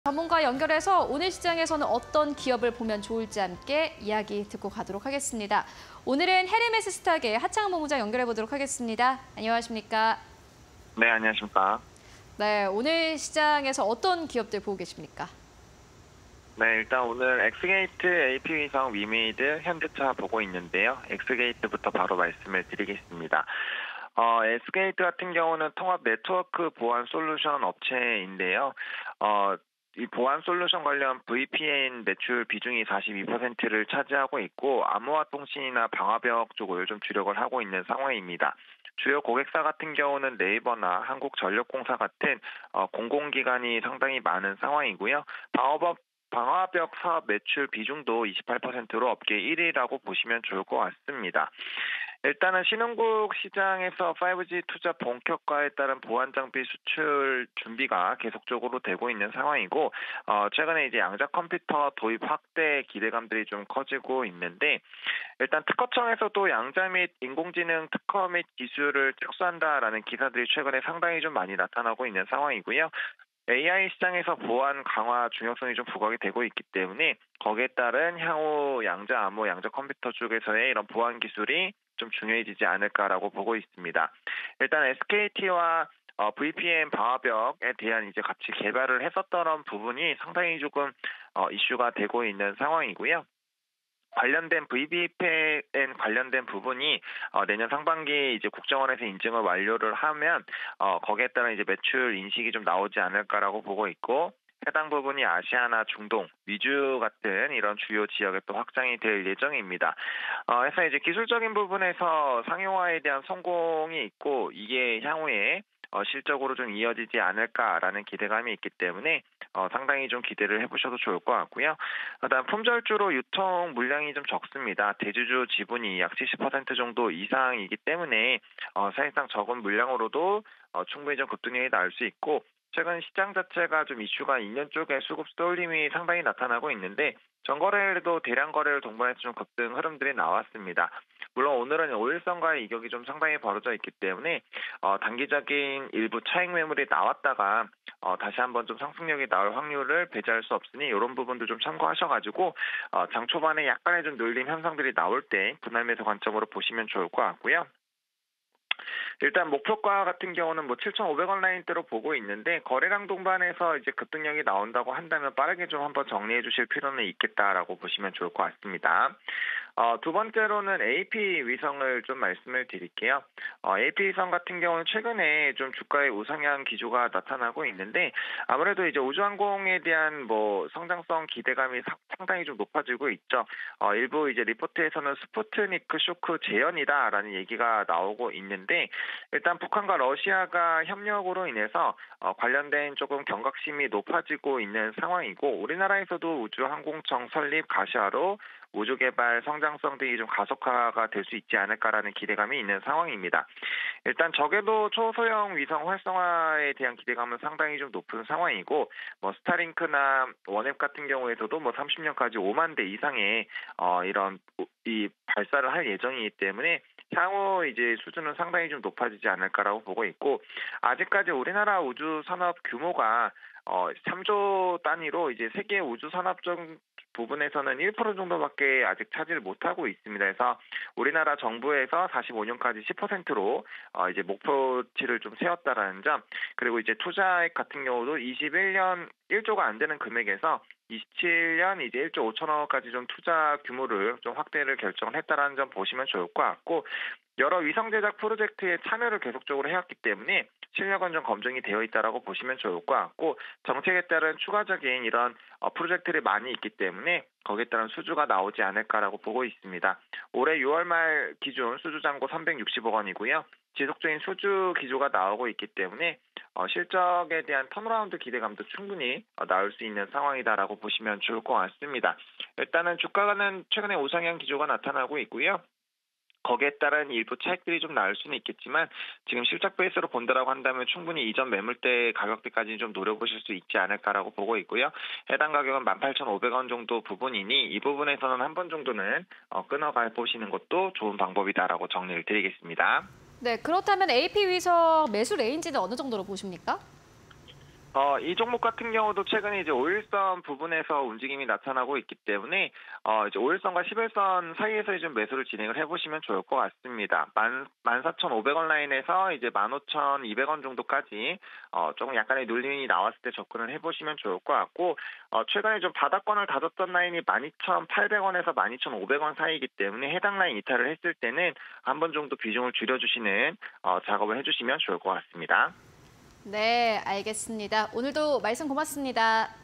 엑스게이트, AP위성, 위메이드, 현대차_하창완의 주력 종목 (20230921)
# 하창완의 주력 종목
1. 엑스게이트(356680)
2. AP위성(211270)
3. 위메이드(112040)
4. 현대차(005380)

엑스게이트(356680)
- 통합 네트워크 보안 솔루션 업체
- VPN 암호화 통신 및 방화벽 관련 제품 주력
- 한국전력공사 등 국가기관 및 NAVER·이마트24 등고객사 확보
- 신흥국 시장, 5G 투자 본격화… 보안 장비 수출 준비
- 홈네트워크 보안 및 양자암호 시장으로 사업 확대
- SKT 등 대기업과 양자암호 시장 진출
- 업계 최초 '퀀텀 VPN' 기술 탑재 제품 상용화
- 아시아·중동·미주 등 진출 예정
- 중동 현지 거점 파트너십 계약 완료
- 대주주 지분 70% 이상… 유통 물량 적어

AP위성(211270)
- 국내 우주 개발 성장성 가속화 기대
- 우주항공청 설립 가시화
- 정찰 위성 등 초소형 위성체계 사업 본격화
- 다양한 위성 제작 프로젝트 참여 따른 수혜 기대
- 북-러 협력으로 '스푸트니크 쇼크' 재현 가능성
- 정부, 韓 우주산업 점유율 2045년까지 10%대 상승 목표
- 우주개발 투자액 2027년 1.5조 규모로 확대